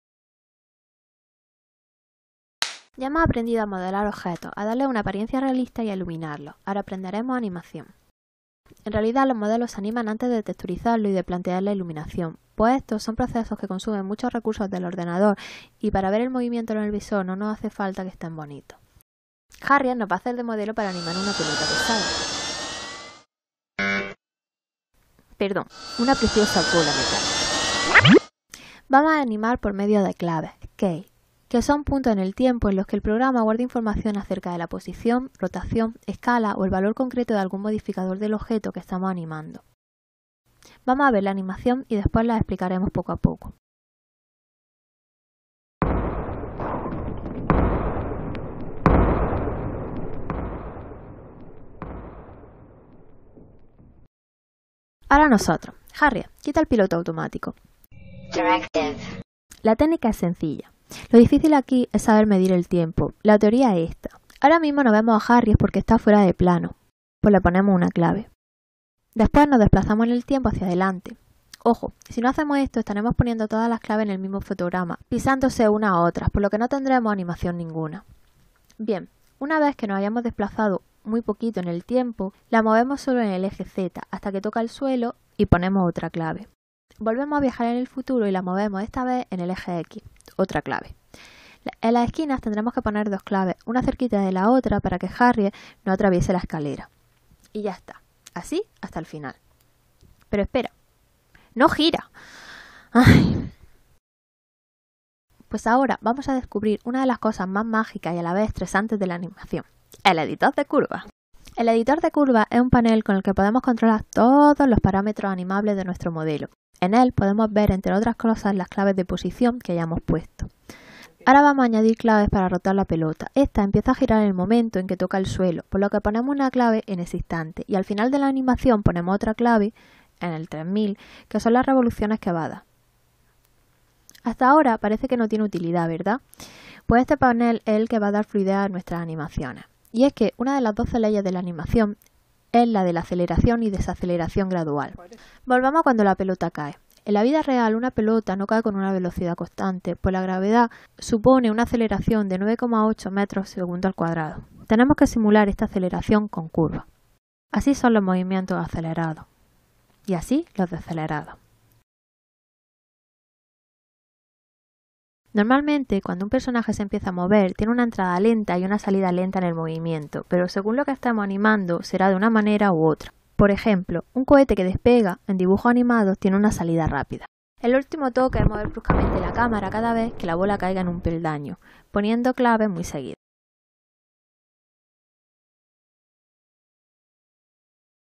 4.2 Animación básica: una bola pesada rodando
Nos iniciamos en la animación por claves con una bola pesada.
Extraído de mi Proyecto fin de carrera: "Creación de tutoriales y videoprocedimientos para la docencia de Modelado 3D y animación por ordenador", Universidad de Granada. 2009/2010.
Modelado y animado en 3dsmax 2009, postpro con Photoshop CS4 y Premiere pro 2.0.